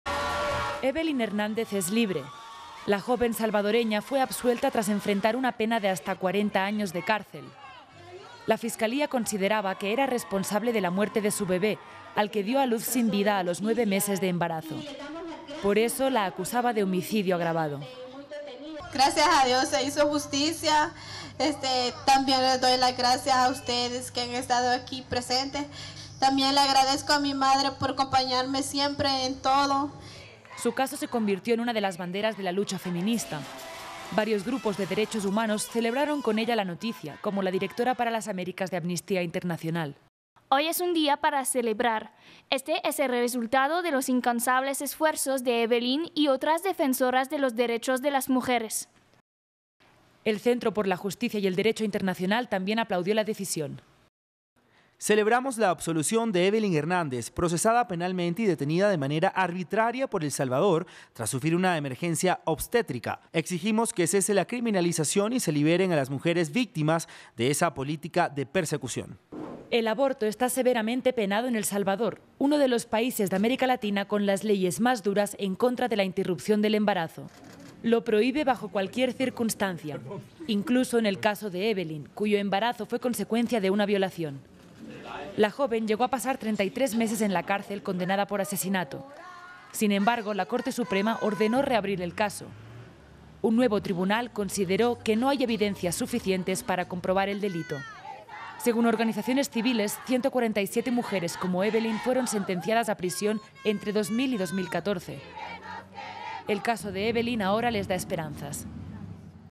El Salvador: Evelyn Hernández, acusada de homicidio por un supuesto aborto fue absuelta
Con una sonrisa y el abrazo colectivo de decenas de personas que la acompañaron a su juicio en el Tribunal de Sentencia de Cojutepeque, en Ciudad Delgado, El Salvador, la joven Evelyn Hernández que fue señalada por un presunto aborto, fue absuelta este lunes 19 de agosto de 2019.